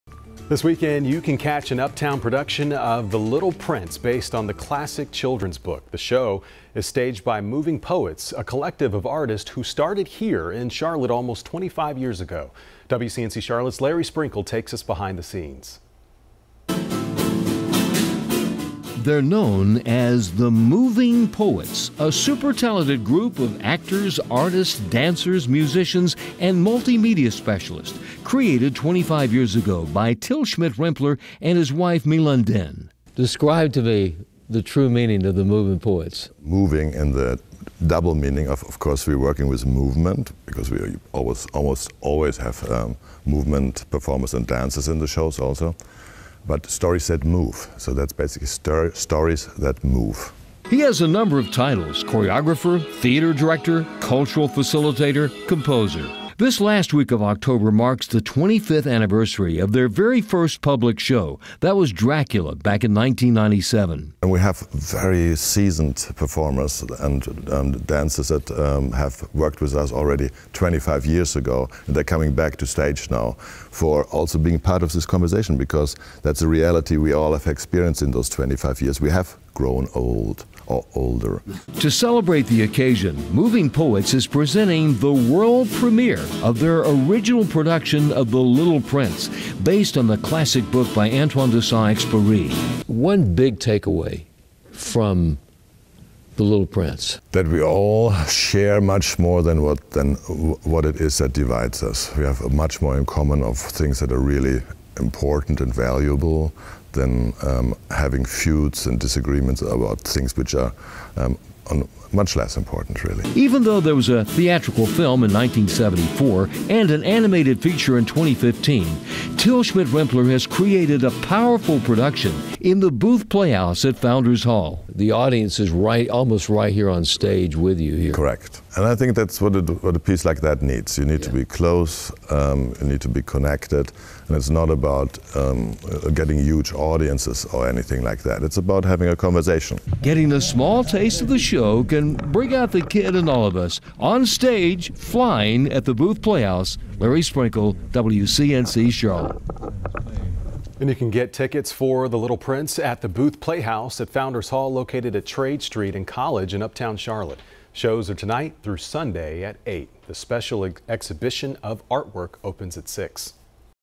Moving Poets celebrates 25th anniversary with special production of 'The Little Prince'
Here in Charlotte, there’s an extremely talented group of actors, artists, dancers, musicians, and audio-video specialists, known as the Moving Poets. Till Schmidt-Rimpler created this troop of consummate performers along with his wife, renowned multidisciplinary artist MyLoan Dinh.

When you have a discussion with Till, you’re talking to a man who has a number of titles: theatre director, choreographer, facilitator, composer, and musician.  You can also add an innovator, educator, and spiritual advisor.  He has spent his life dedicated to the arts.